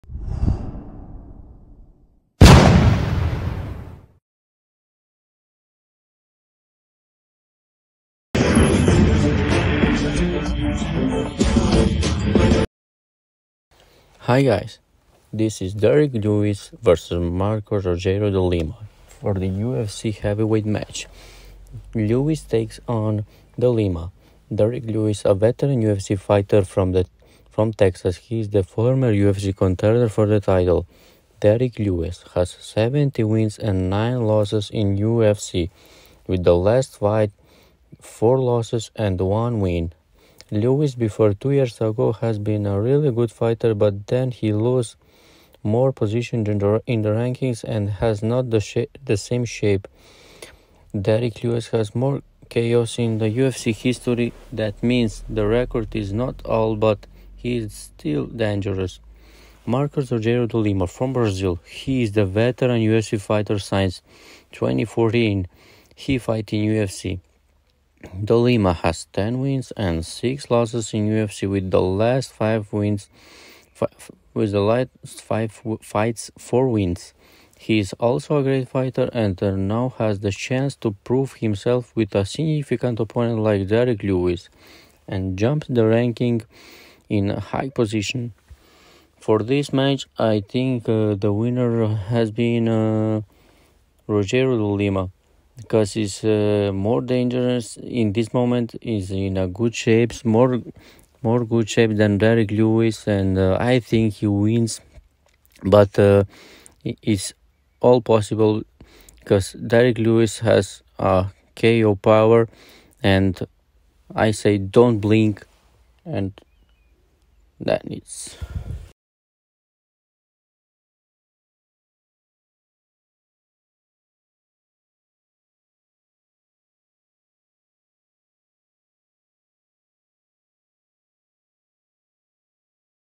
Derrick Lewis vs Marcos Rogerio De Lima | Highlights before the match
Derrick Lewis vs Marcos Rogerio De Lima

UFC heavyweight match

Derrick Lewis a veteran ufc fighter from Texas he is the former ufc contender for the title. Derrick Lewis has 17 wins and 9 losses in ufc with the last 5 fights 4 losses and 1 win. Lewis before two years ago has been a really good fighter but than he lose more position in the rankings and has not the same shape. Derrick Lewis has more KO’s in ufc history that means the record is not all but he is still dangerous.

Marcos Rogerio De Lima from Brazil, he is the veteran UFC fighter since 2014 he fight in ufc. De Lima has 10 wins and 6 losses in ufc with the last 5 fights 4 wins. He is also a great striker and now has the chance to prove him self with a significant opponent like Derrick Lewis and jump the ranking in a high position.